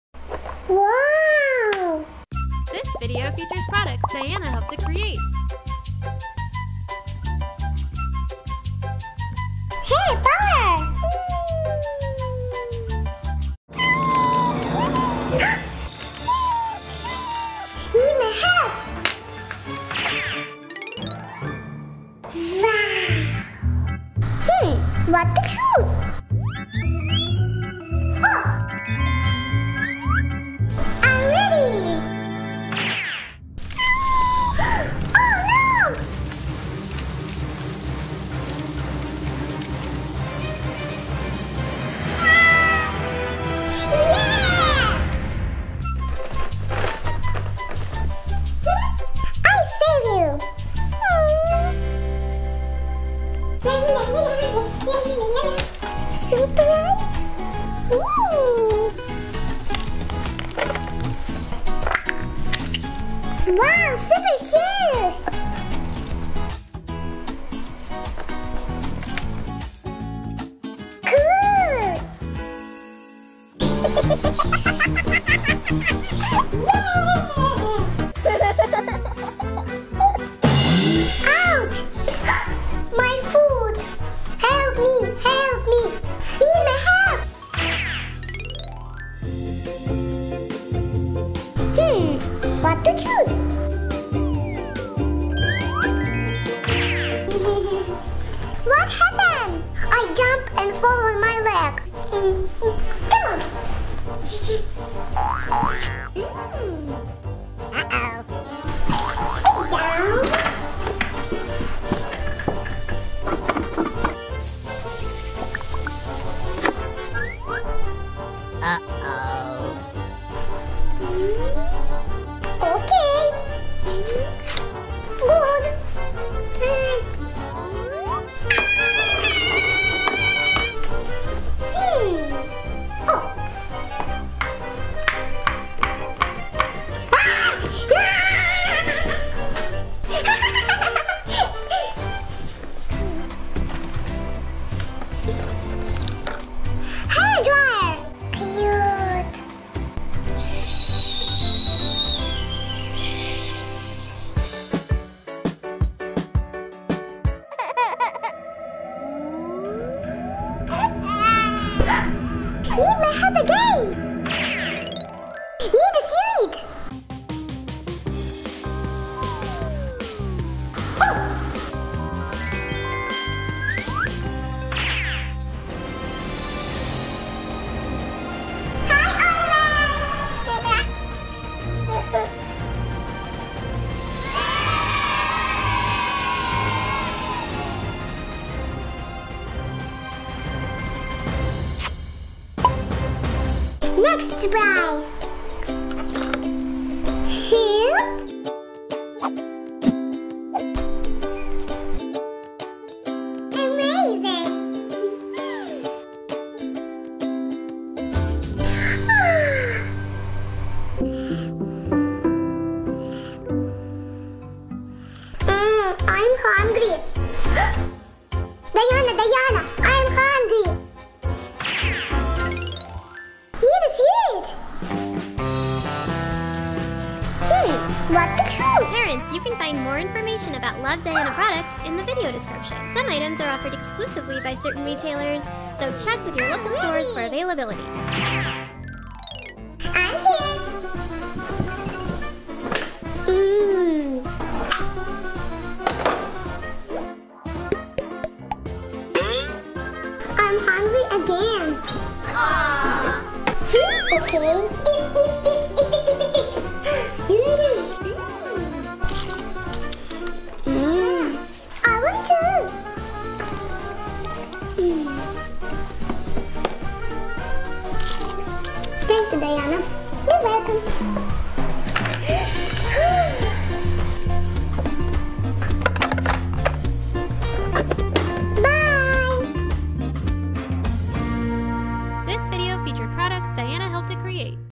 up as _ Superheroes and helps her family "when they need help"
Parents, find more information about the Love, Diana Hair Power Mini Mystery Shopper
Diana plays with the new Hair Power Mini Mystery Shopper set. She sees that her family needs help. The girl dresses up as a superhero and helps her family.